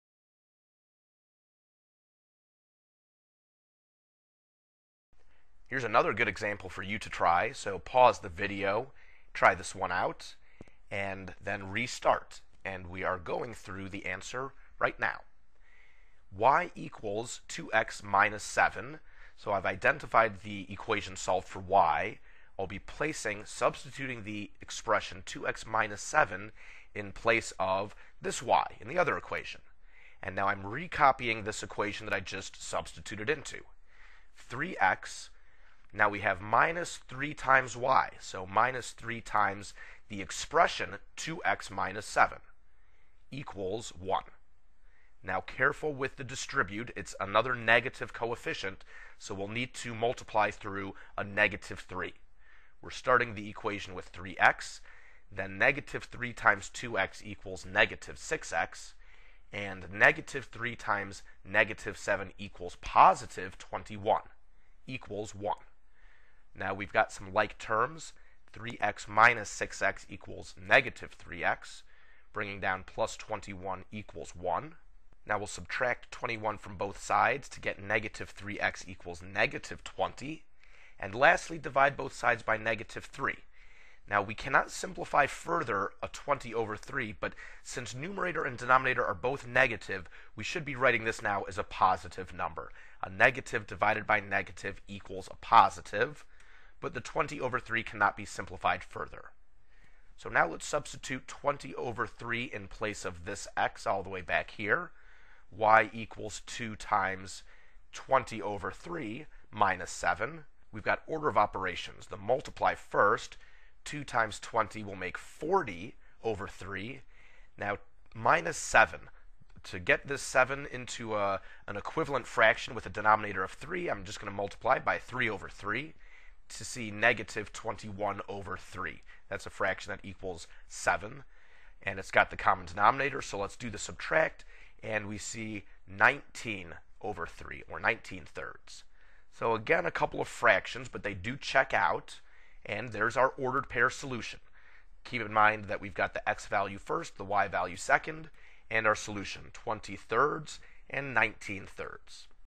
Solving Systems of Linear Equations by Substitution: Substituting with a Negative Coefficient (9/12)
Here's a breakdown of the original video into separate pieces. This series shows the ideas and procedures behind solving systems of linear equations by substitution. Grab a pen and paper and work along with the videos. Remember, you can pause and rewind whenever you want! :·⌡